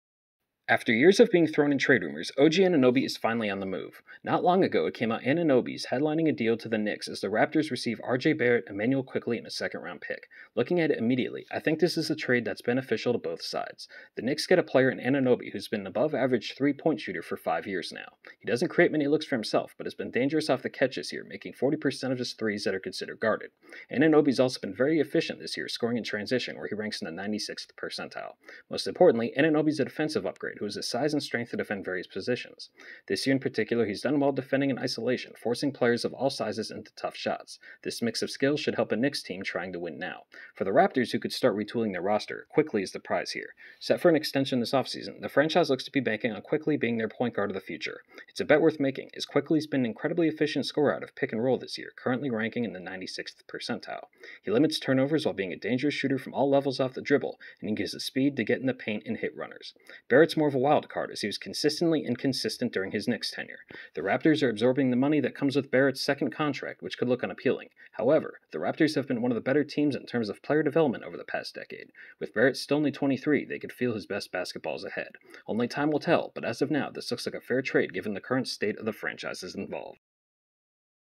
OG Anunoby trade reaction
A quick reaction to the Toronto Raptors sending OG Anunoby to the New York Knicks and how it could be beneficial for both sides